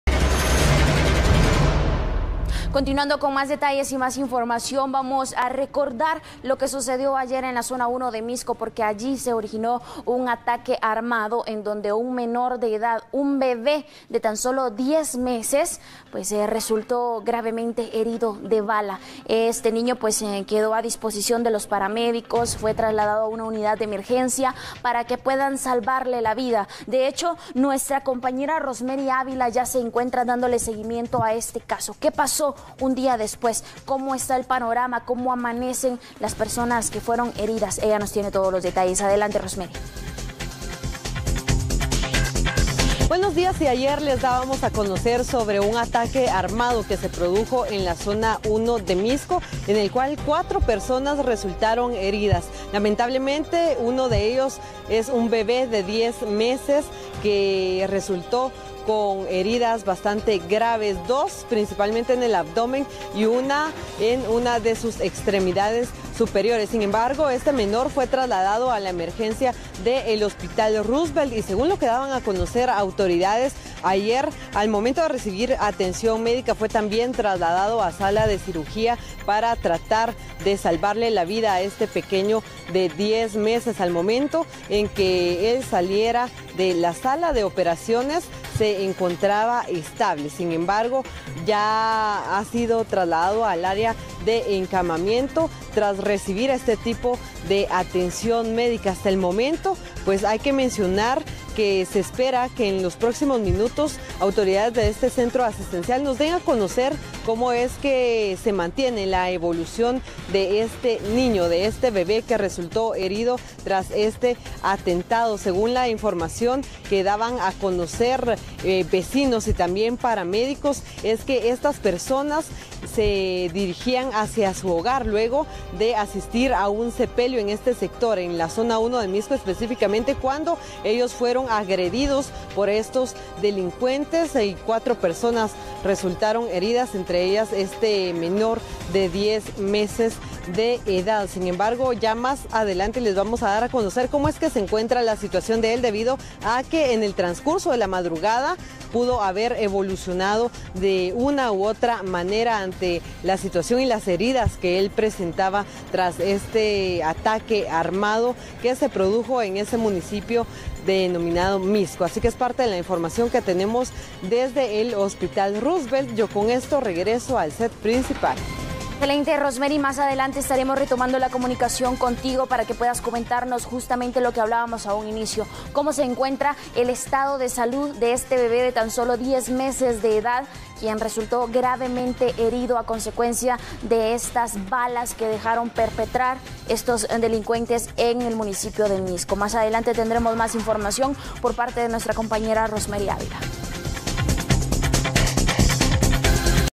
Un bebé de 10 meses fue herido de bala
La víctima fue trasladada de emergencia al Hospital Roosevelt tras resultar gravemente herido. El ataque armado registrado en la zona 1 de Mixco dejó cuatro personas heridas de bala.